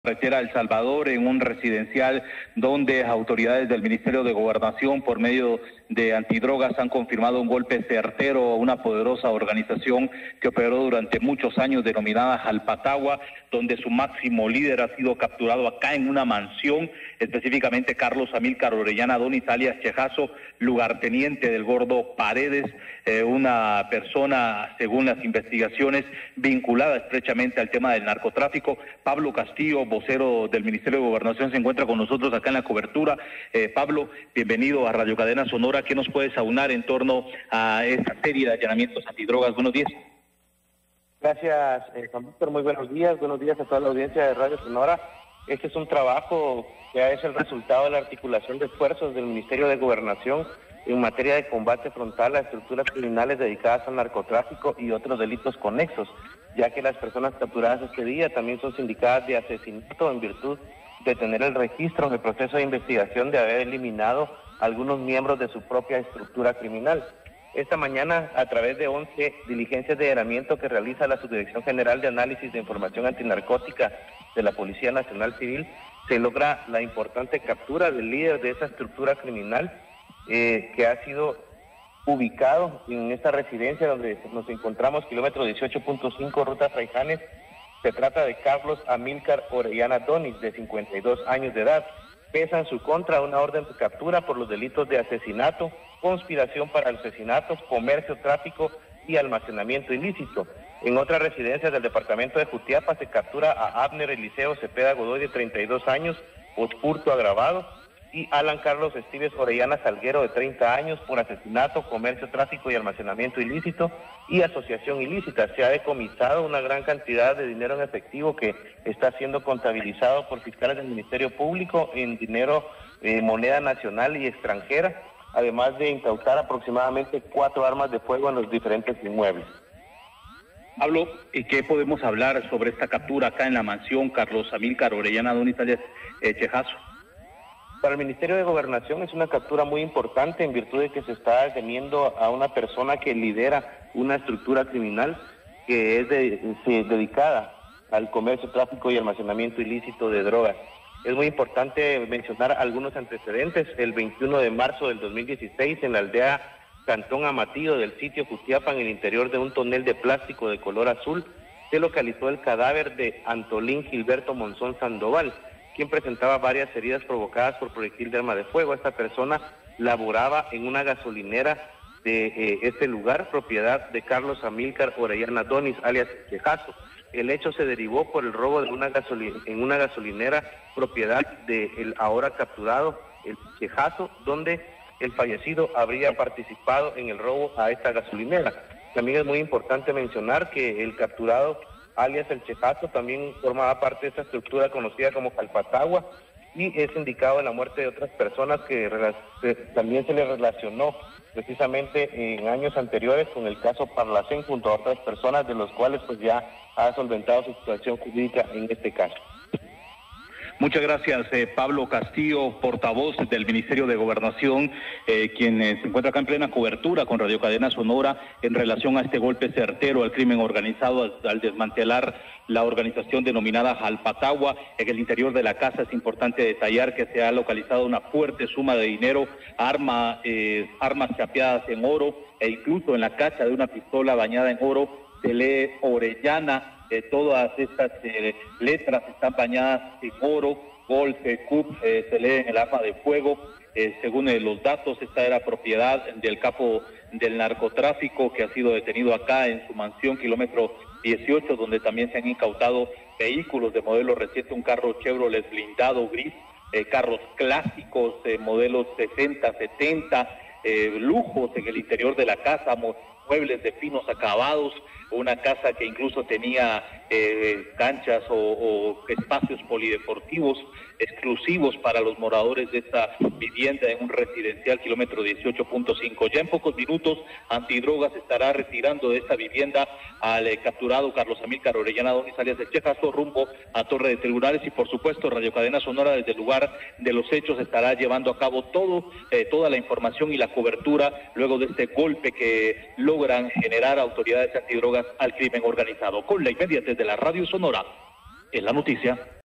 Desmantelan la organización criminal denominada ‘Jalpatagua’
El Ministerio de Gobernación en coordinación con Antinarcóticos ha logrado desmantelar esta organización criminal.